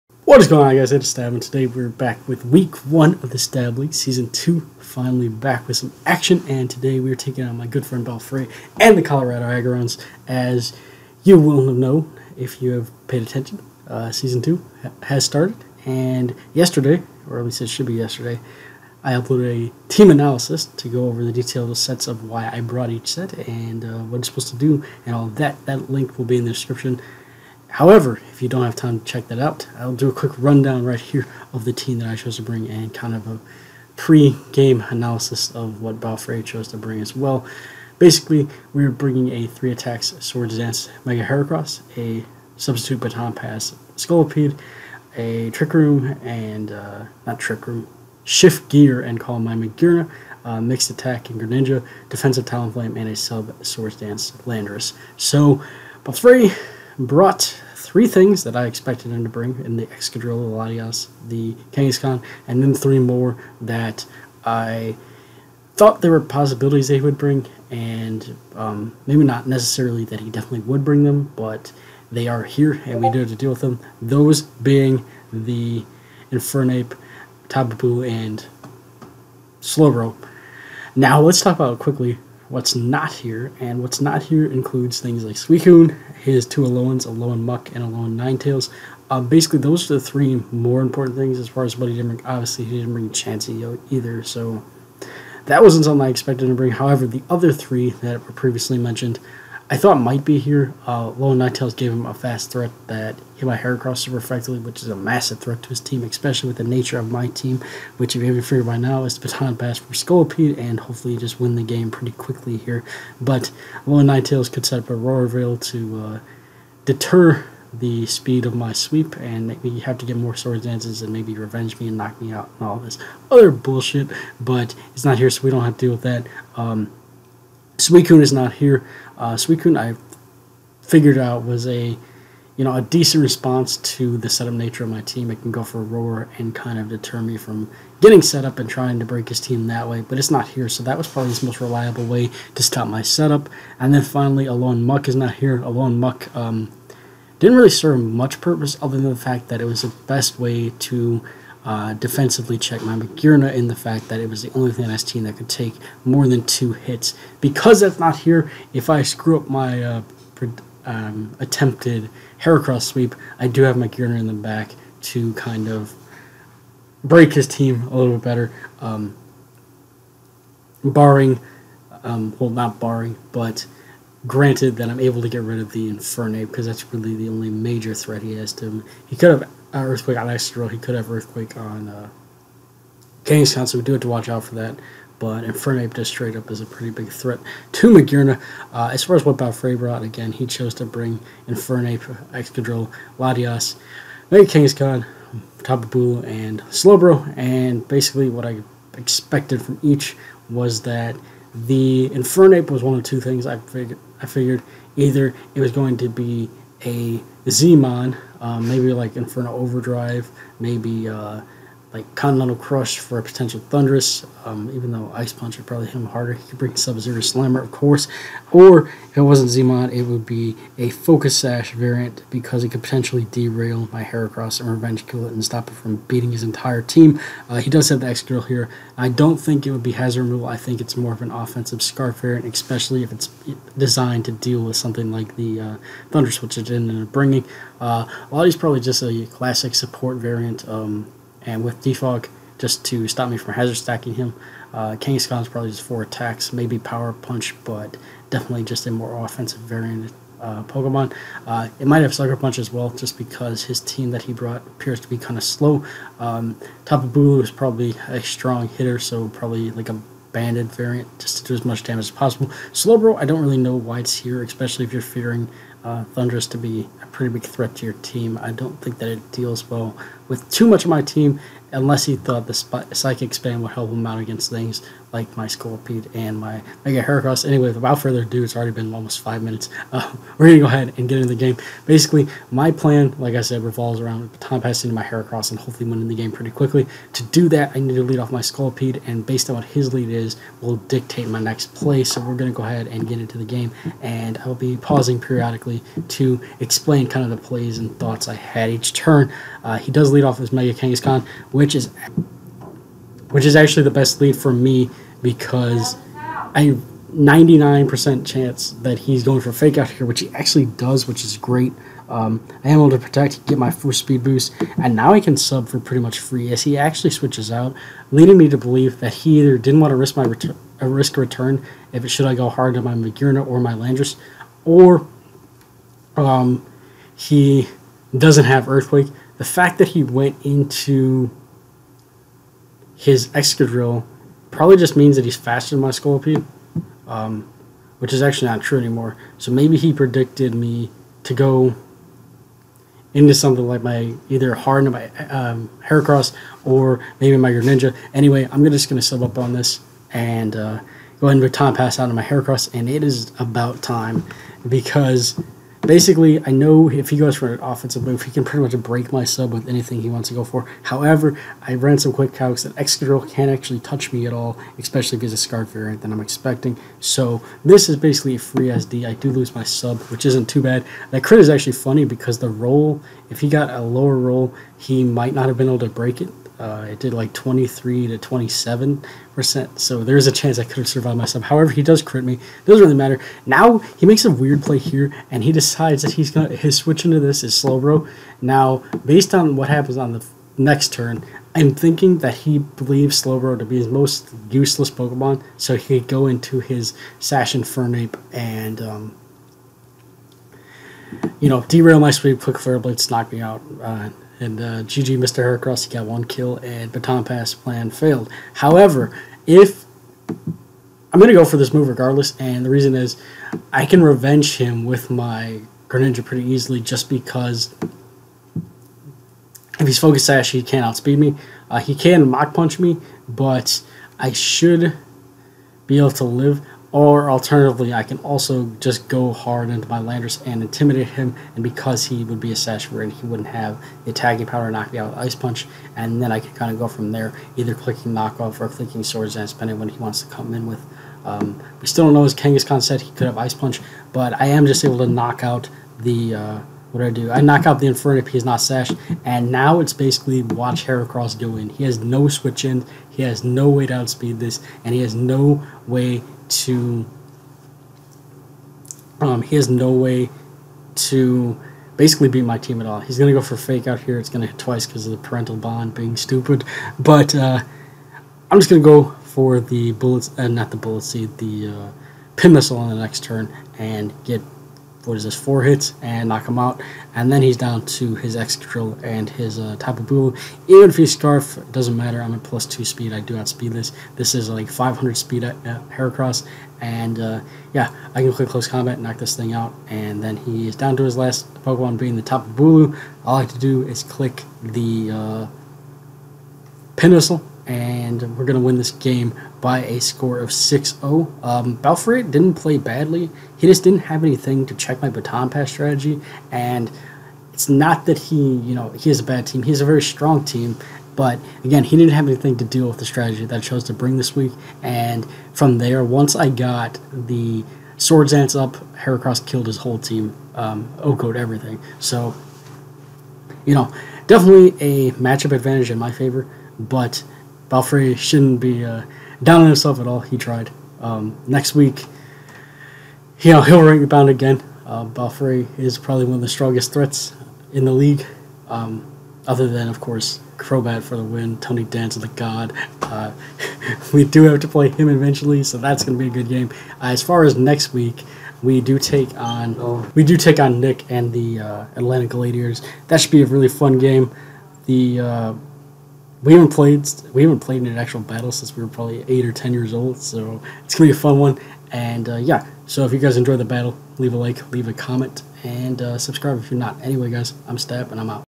[STAB League S2W1] Chicago Sacred Fire Vs Colorado Aggrons " Bypassing The Wrath of KangasKHAN"
Whats going guys? its STAB and today we kick off STAB League Season 2 with a week 1 rematch of last season's championship taking on Balfre and the Colorado Aggrons!